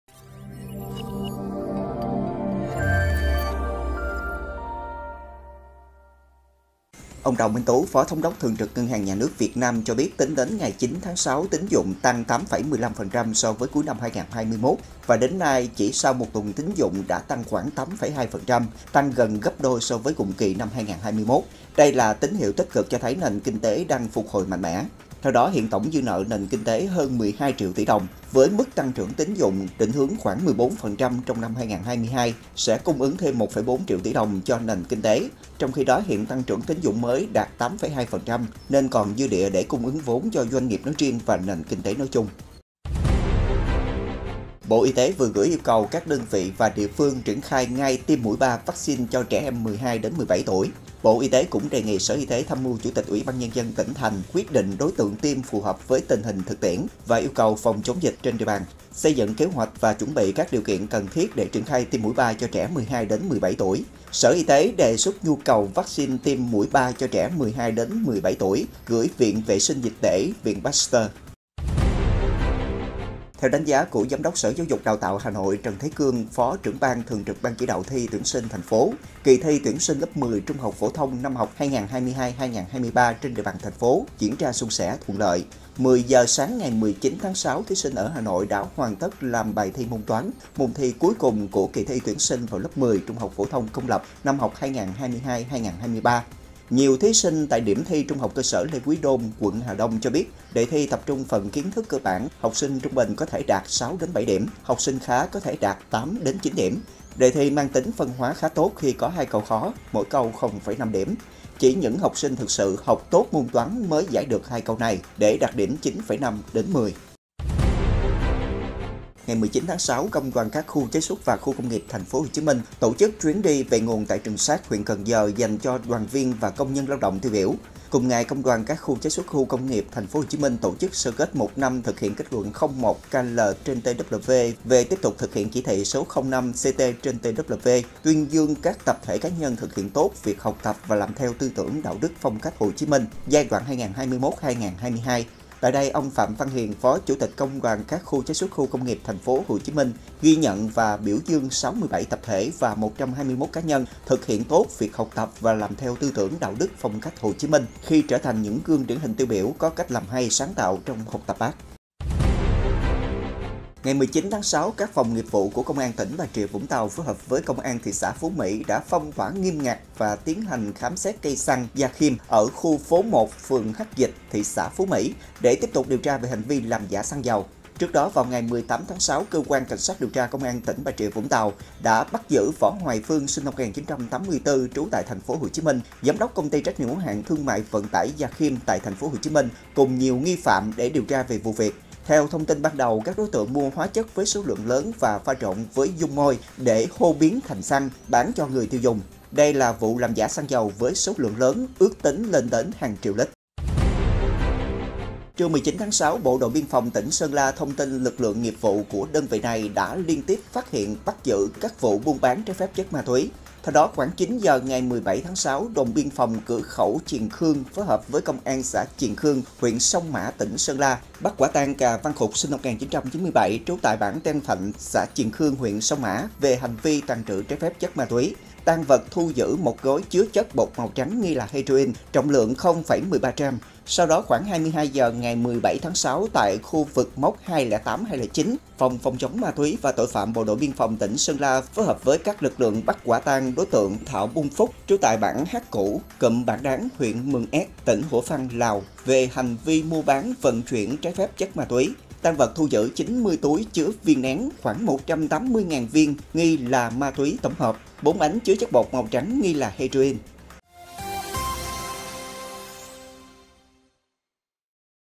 Điểm tin SGGP Online ngày 19-6-2022
Triển khai tiêm vaccine Covid-19 mũi 3 cho trẻ 12-17 tuổi; Khám xét một cây xăng lớn ở Bà Rịa – Vũng Tàu; Bắt quả tang thanh niên vận chuyển 4 bánh heroin và 180.000 viên ma túy tổng hợp… là những nội dung đáng chú ý có trong Điểm tin SGGP Online ngày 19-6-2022.